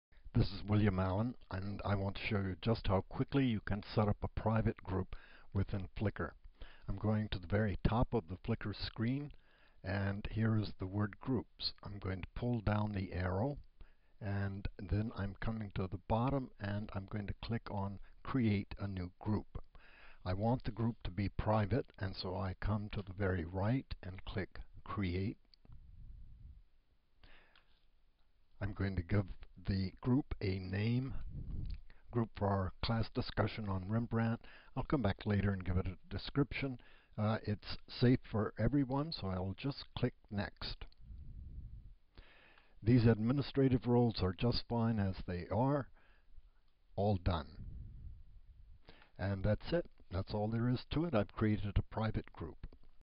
Create A Private Discussion Group In Flickr
Demonstrates how to easily create a private discussion group in Flickr; advantage is inclusion of rich media